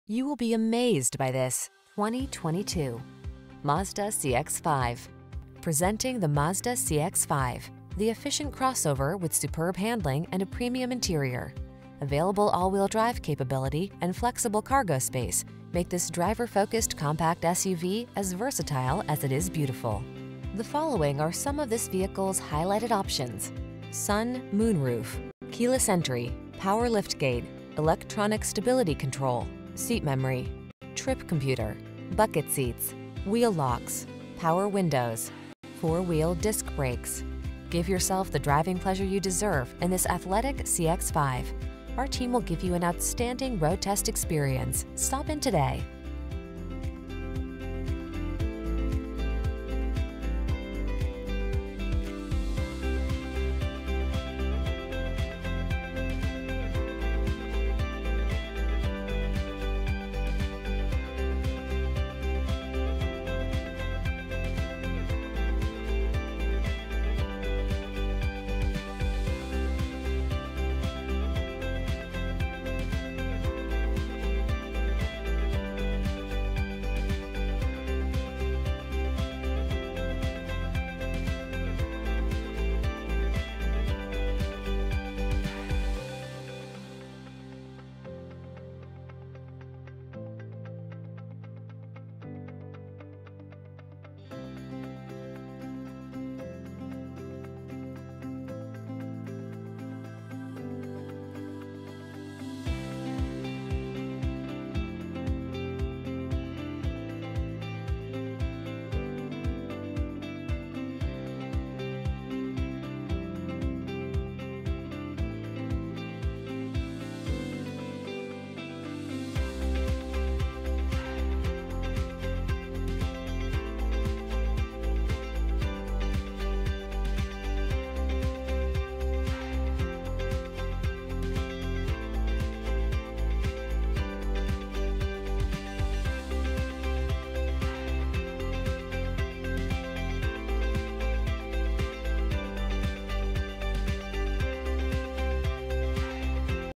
2022 Mazda CX-5 Milford, Danbury, Stamford, New Haven, Hamden 322083
Eternal Blue Mica New 2022 Mazda CX-5 available in Milford, Connecticut at Mazda of Milford. Servicing the Danbury, Stamford, New Haven, Hamden area.
2022 Mazda CX-5 2.5 S Premium Package - Stock#: 322083 - VIN#: JM3KFBDM7N1534413

Eternal Blue Mica 2022 Mazda CX-5 2.5 S Premium Package AWD 6-Speed Automatic 2.5L I4 DOHC 16VbrbrRecent Arrival!